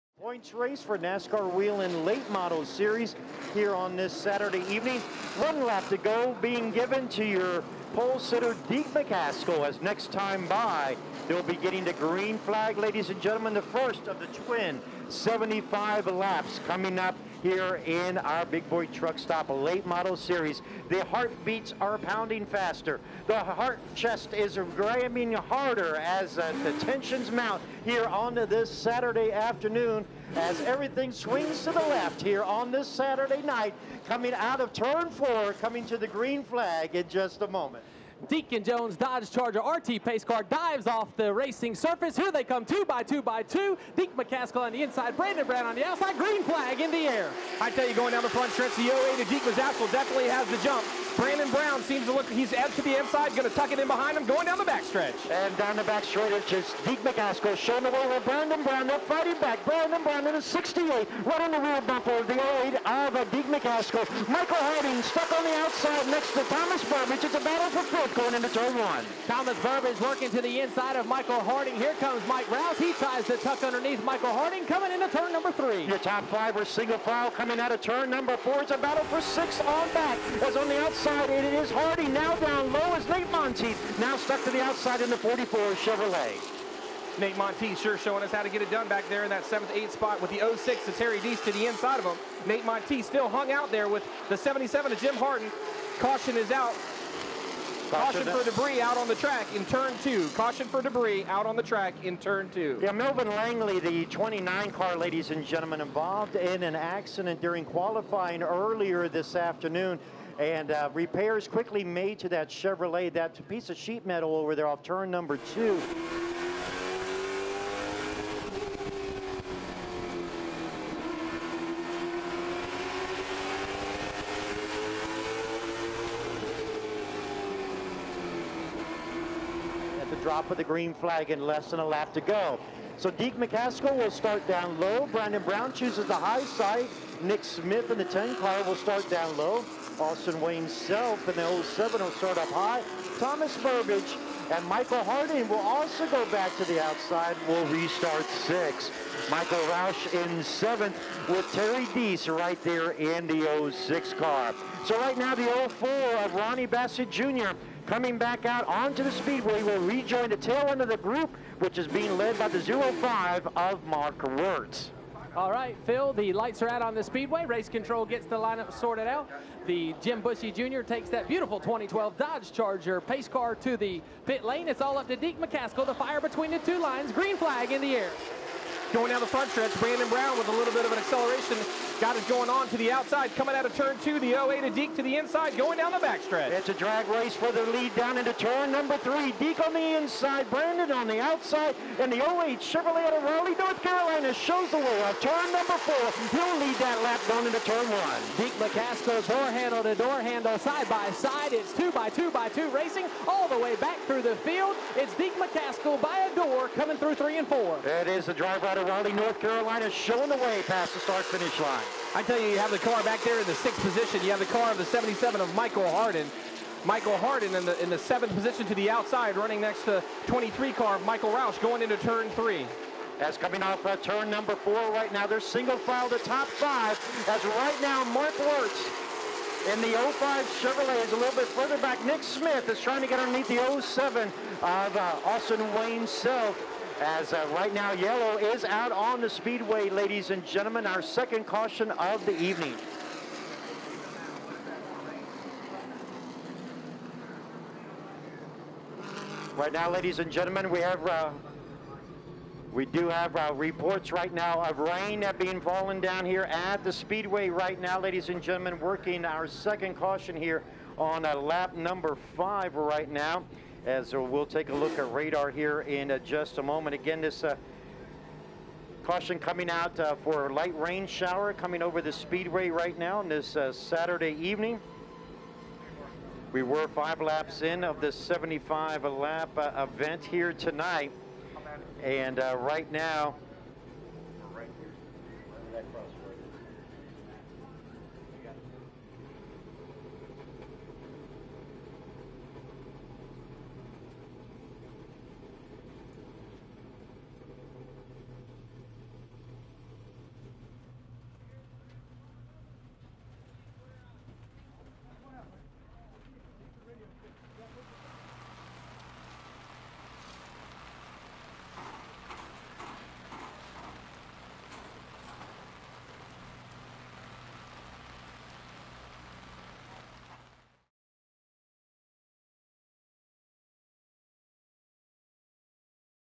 Southern National - 9/8/12 - LMSC - 75 Lap Feature #1 - "Full Race"
The whole Late Model Stock Car race at Southern National Motorsports Park on September 8, 2012 before the rain came.

The recorded PA Audio isn't perfect, they were having some kind of audio problems that night that lead to it to be too high of a level, typically the PA Audio on the DVDs is quite clear.